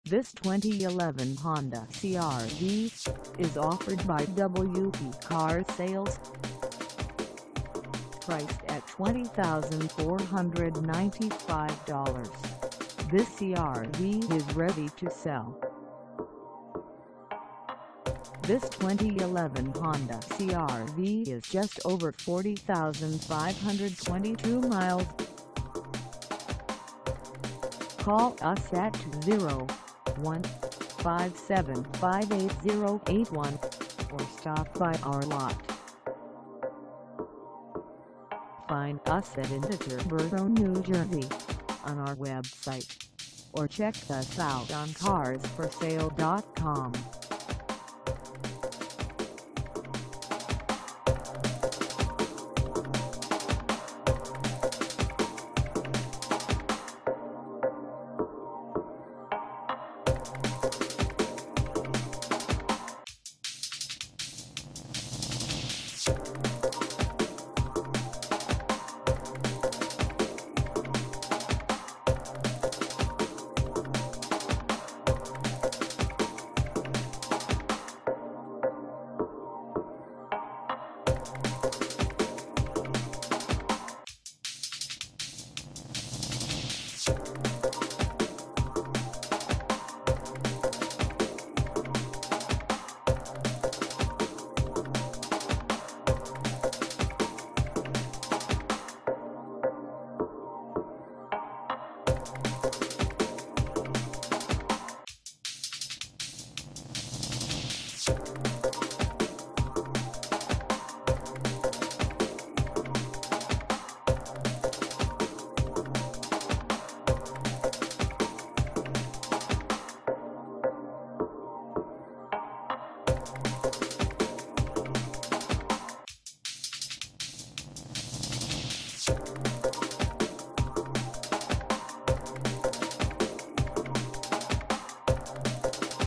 2011 Honda CR-V SE 4WD 5-Speed AT for sale in Teterboro, NJ
This 2011 Honda CR-V SE 4WD 5-Speed AT is for sale in Teterboro, NJ 07608 at  WP Car Sales  .
Engine: 4-Cylinder L4, 2.4L
Transmission: Automatic
Color: Gray
Mileage: 40522
Contact  WP Car Sales    Teterboro, NJ 07608 to test drive this 2011 Honda CR-V SE 4WD 5-Speed AT today.